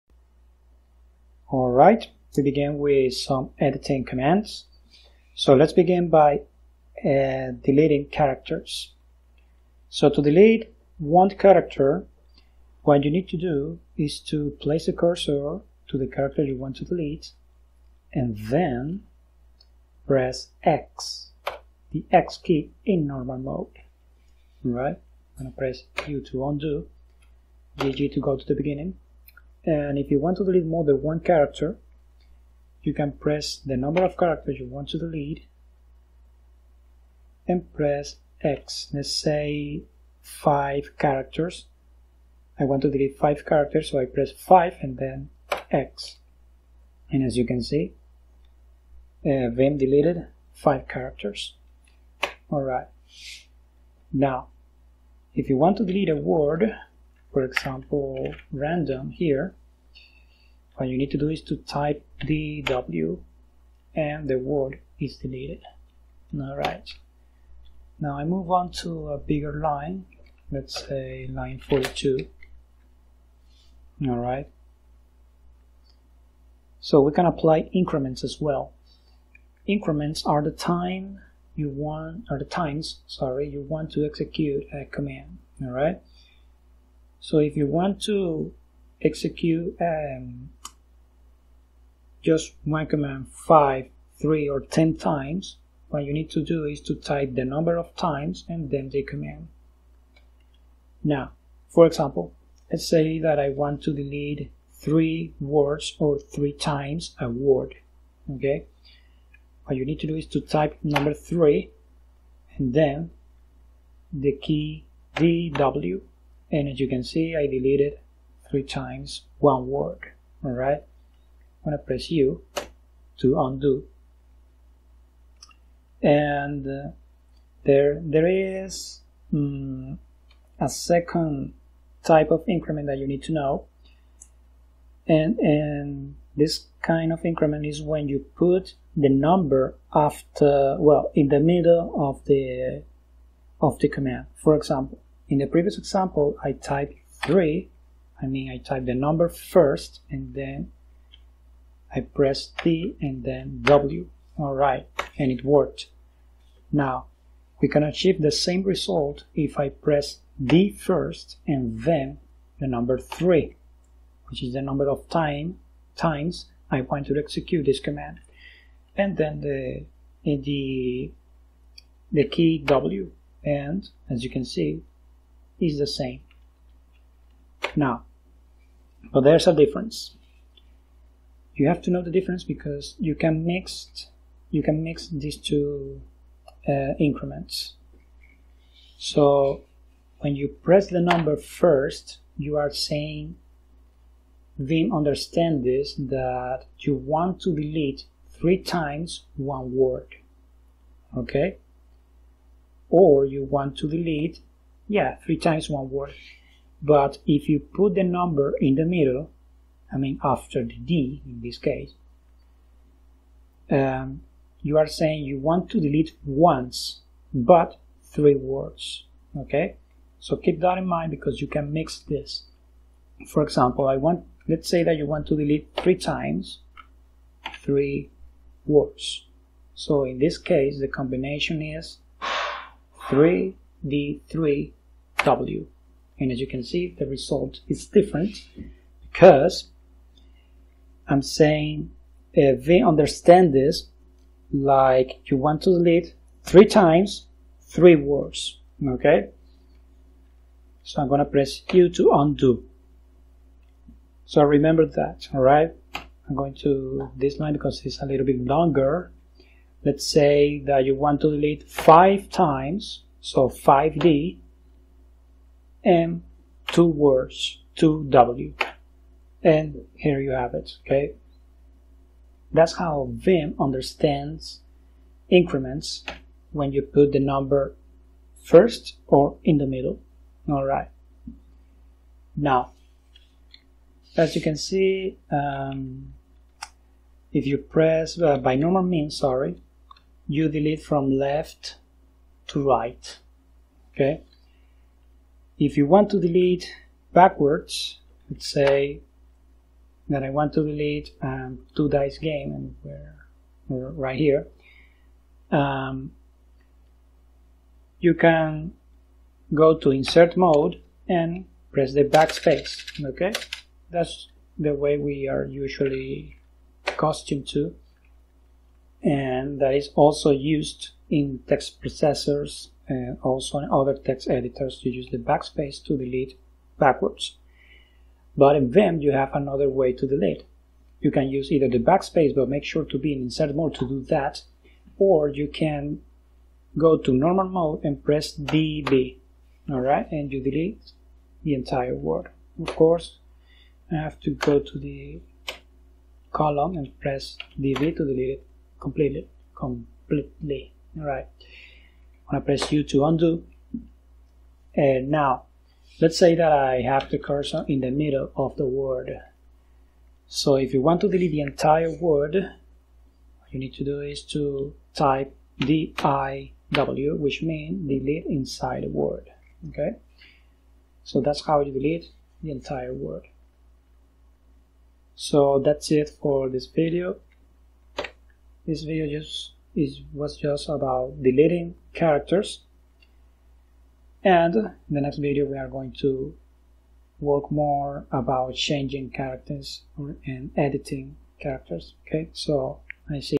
Deleting Characters in Vim
You can use the backspace key to delete characters in Vim, but this only works if you are in Insert Mode. Most deleting commands are executed in Normal Mode.
There's an important difference between 3dw and d3w. Vim understands the first command as "delete one word three times" and the second command as "delete three words once". These two commands can be mixed as "3d3w" which means "deletes three words three times". It may be confusing at the beginning but it is just a matter of practice.
In case you want to delete a word, make sure to place the cursor at the beginning or at the end of the word you want to delete, you can even place the cursor in the middle of the word you want to delete, make sure to execute the right for the right situation.